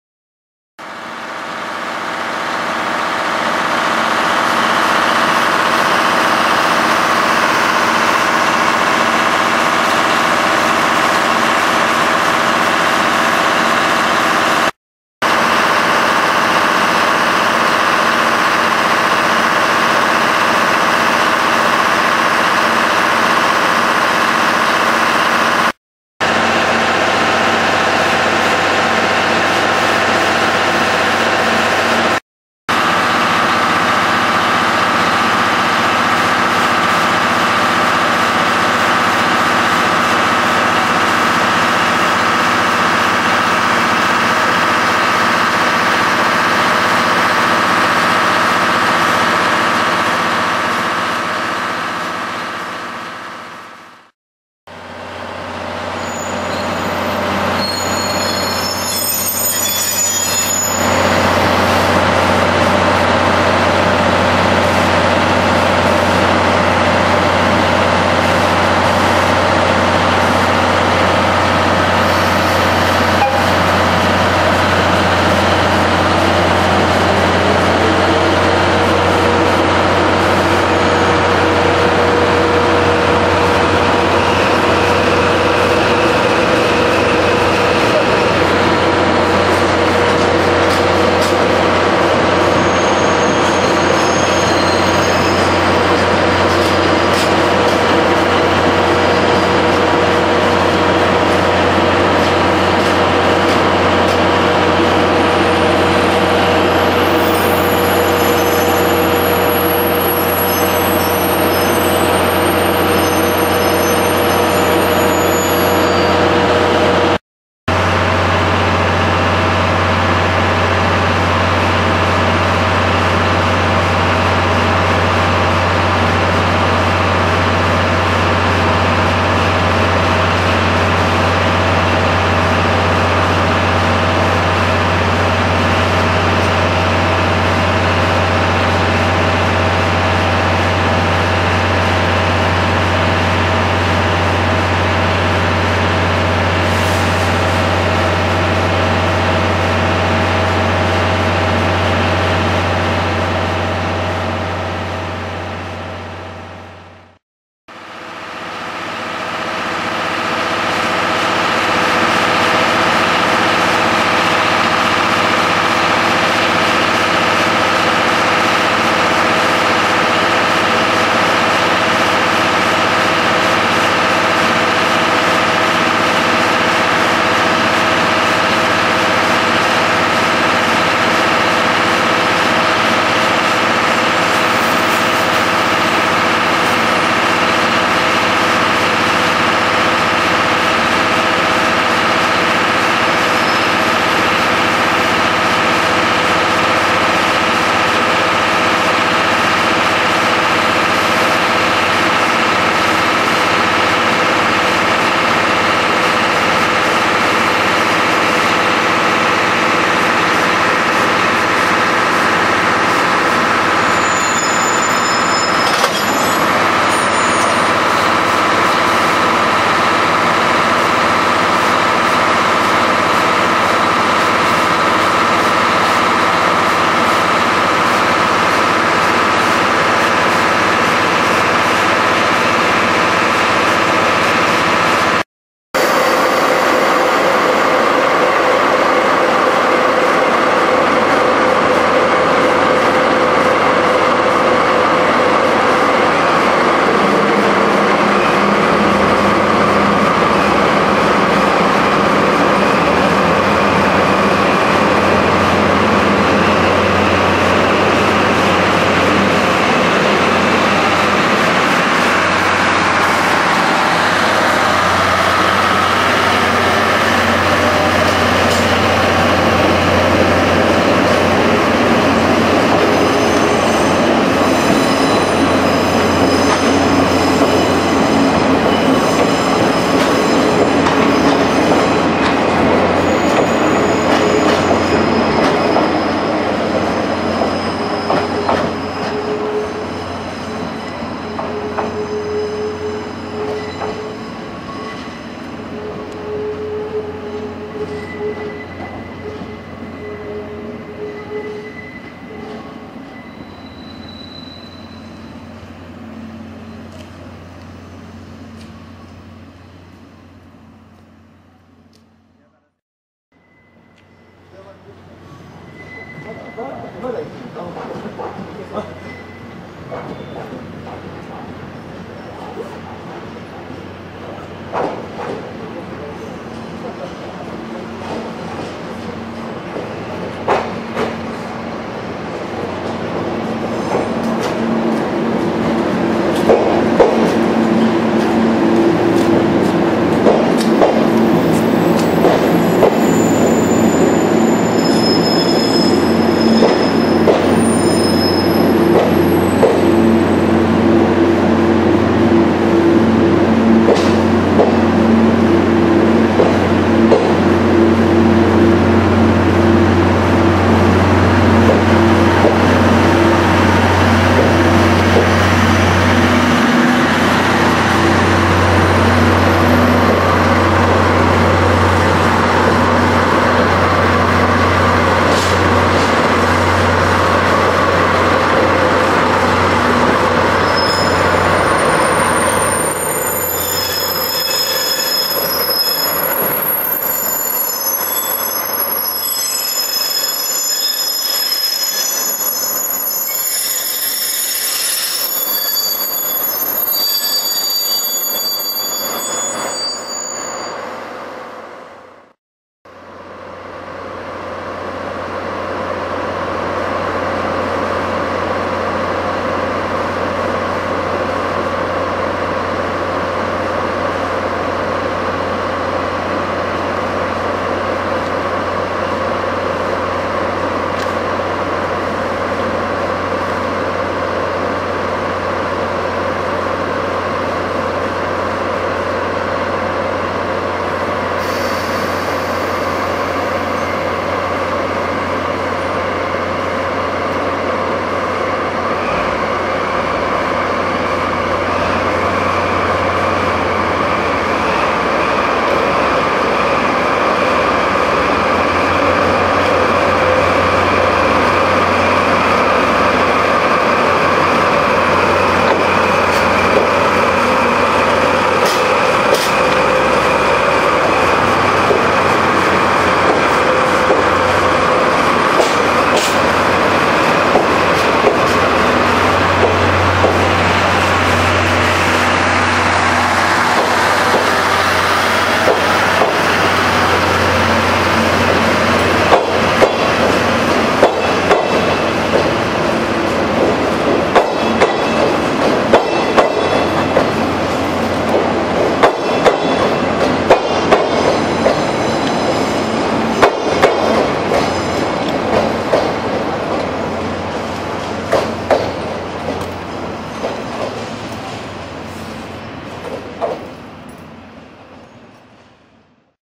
深夜の米子駅
12系を使用したハンドル訓練は深夜帯にも実施されました。出区から発車までの映像です。